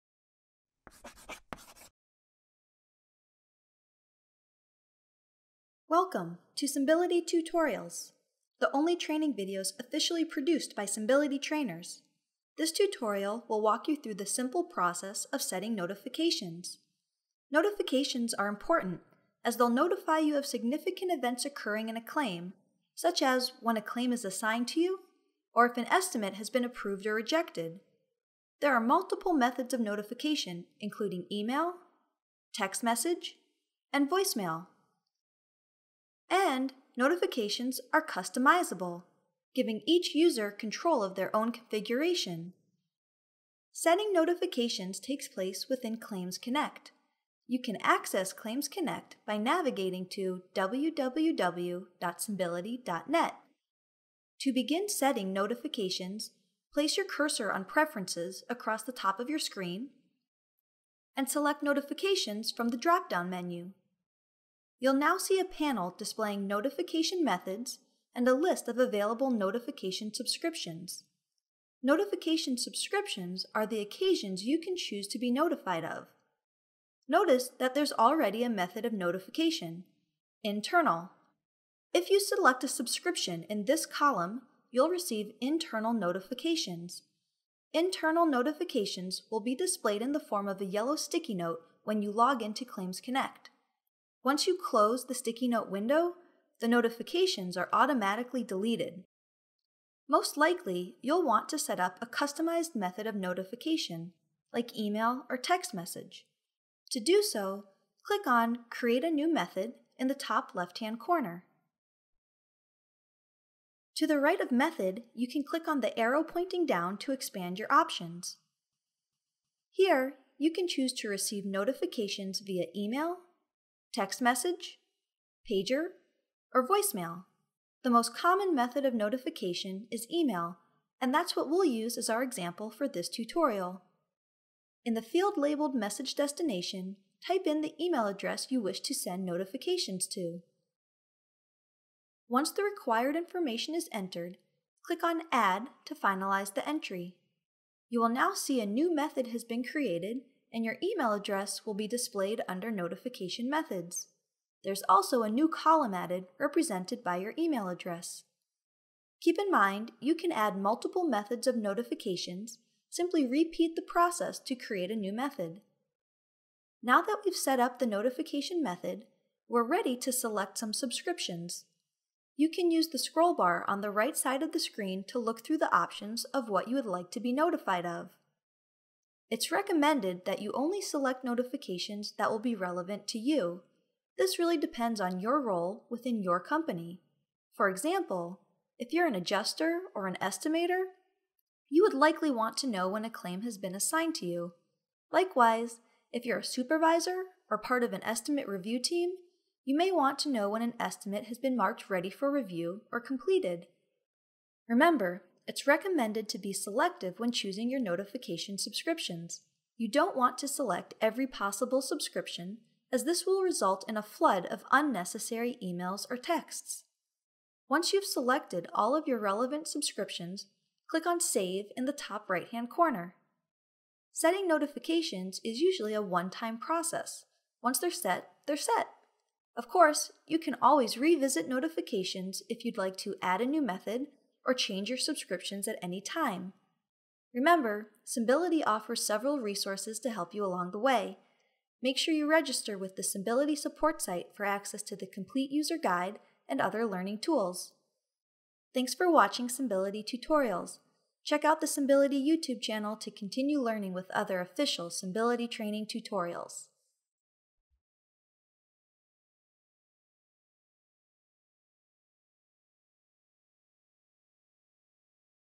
Setting Notifications on Symbility Claims Connect
This training video will guide you through how to set up notifications on Symbility Claims Connect, so you won't miss any important claim changes and updates.